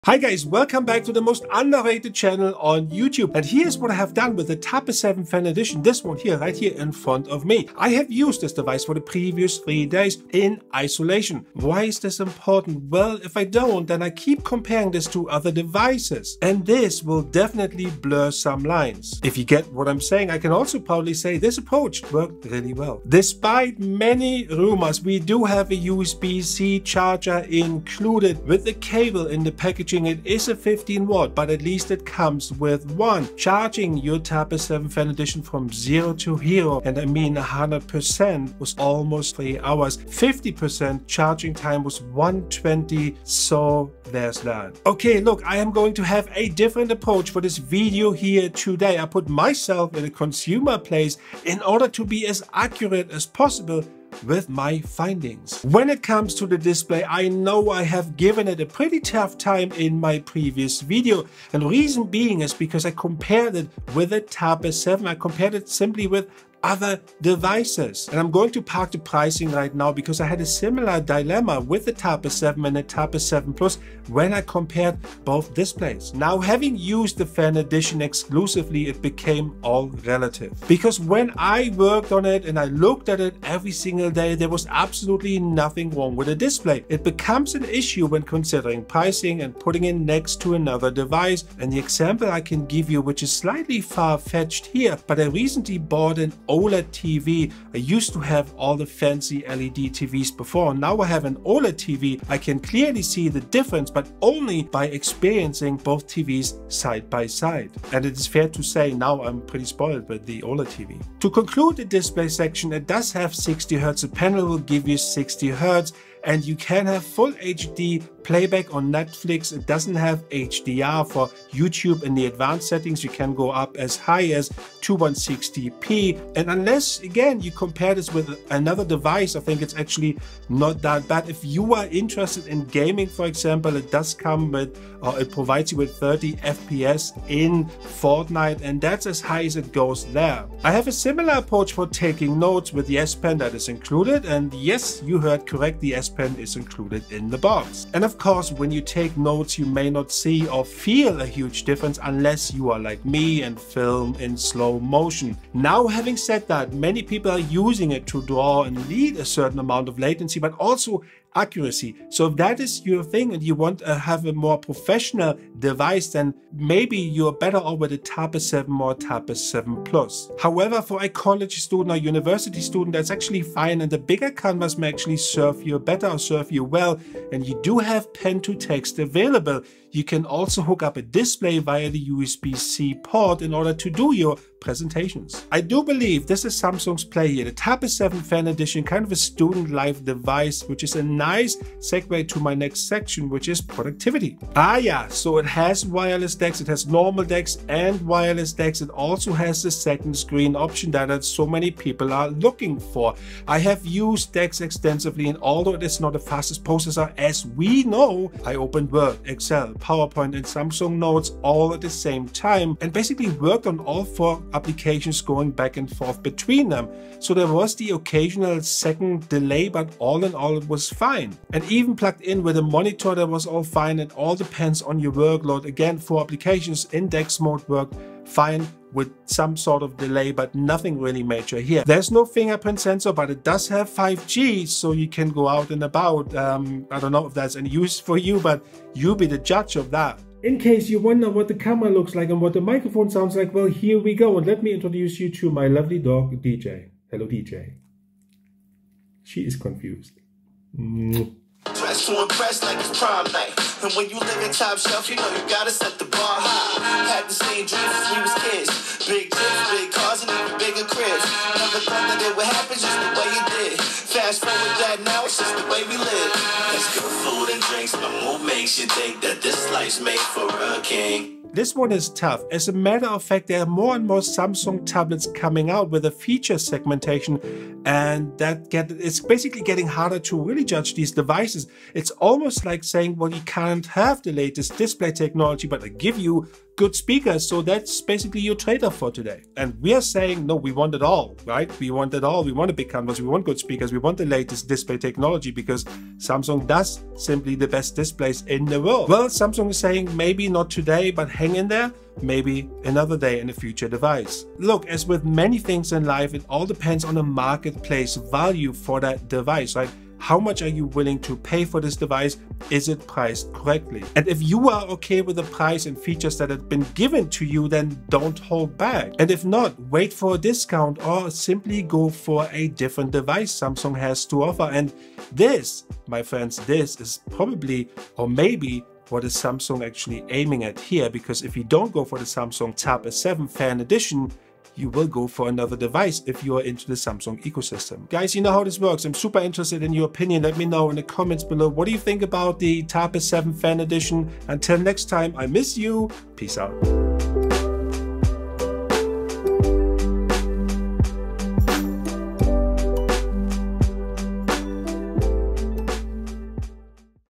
NEW Galaxy Tab S7 FE - Powerful Enough For Productivity? (REVIEW)
I am looking at the Samsung Galaxy Tab S7 FE to see if it has enough power to handle DeX with heavy-duty tasks. Can the Samsung Galaxy Tab S7 FE actually go through this without any issues? Are you looking to buy a Samsung Galaxy Tab S7 FE?